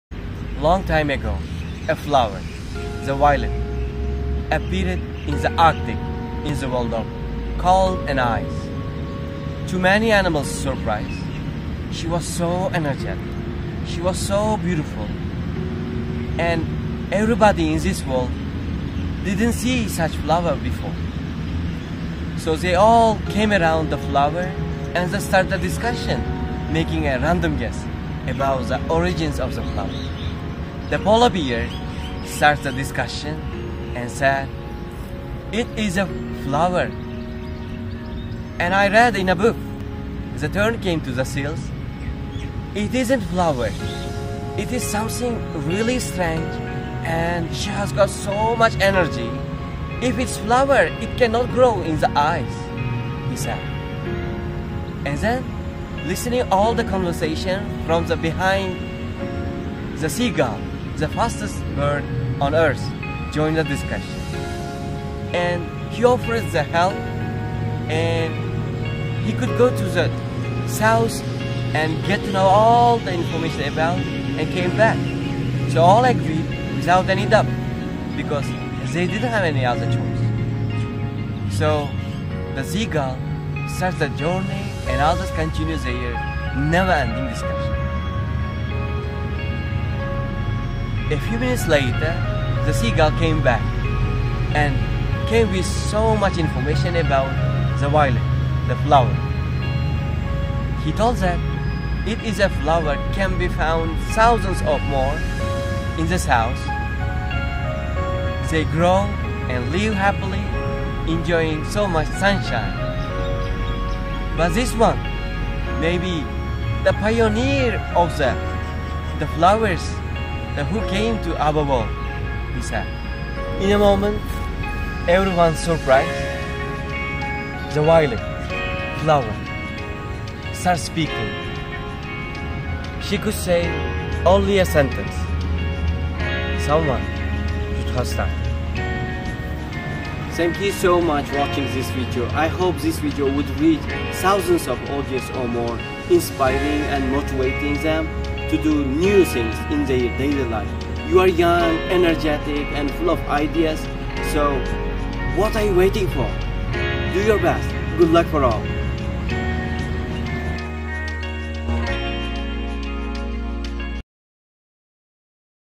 Story about the violet! Someone should start! Motivational Video by Jaloliddin Kosimov.
Story about the violet! Someone should start! Motivational Video by Jsloliddin Kosimov.